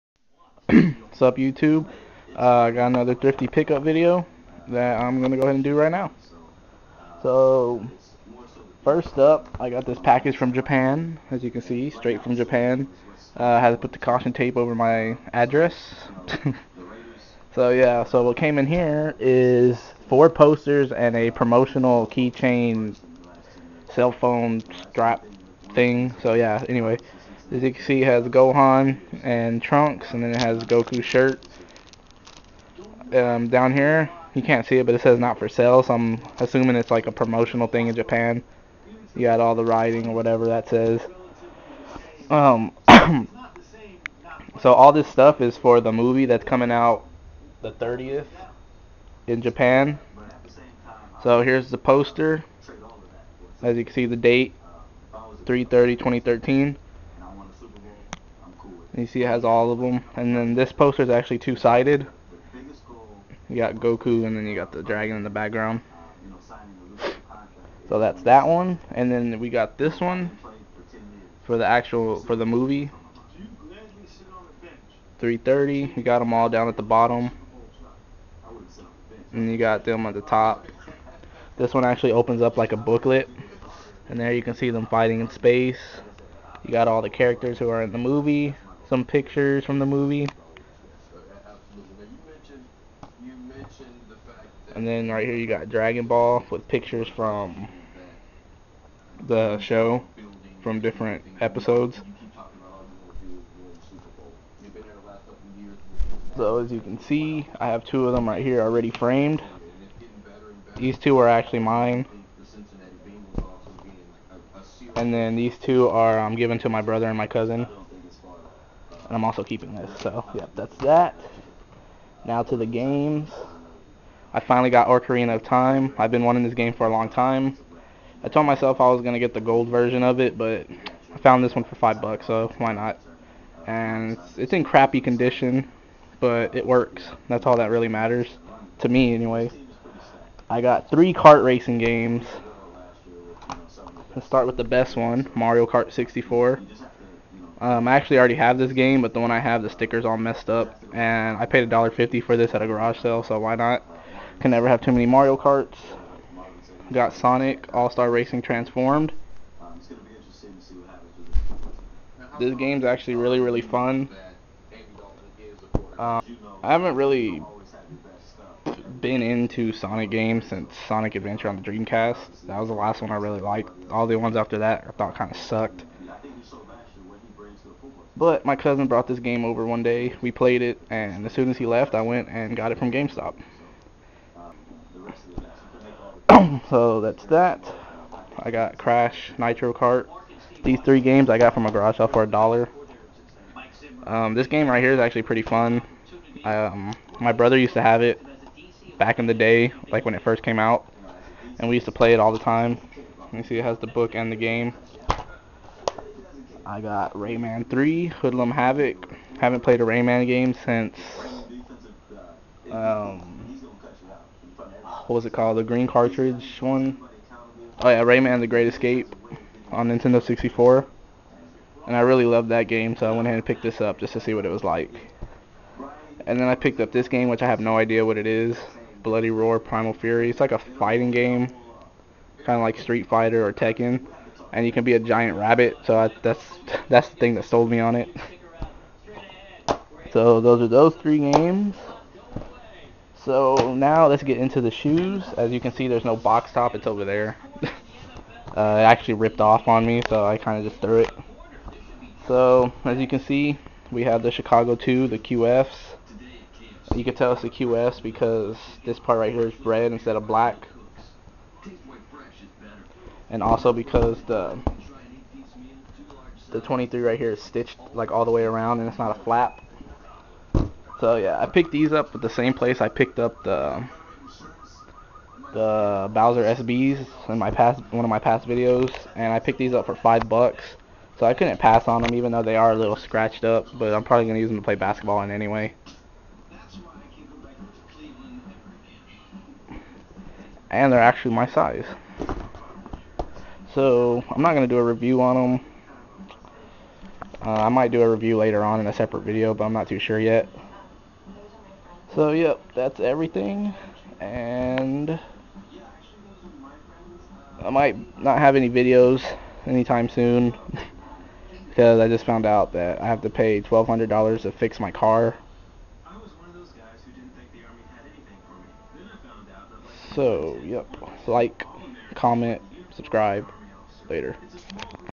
Thrift Store Pick Ups #10 - Air Jordan Chicago 2, N64 Ocarina Of Time, Dragonball Z Battle Of Gods
Got some Chicago 2's, Dragonball Z Battle Of Gods Poster's from Japan, Ocarina Of Time, Mario Kart 64, Sonic Racing Transformed, and more.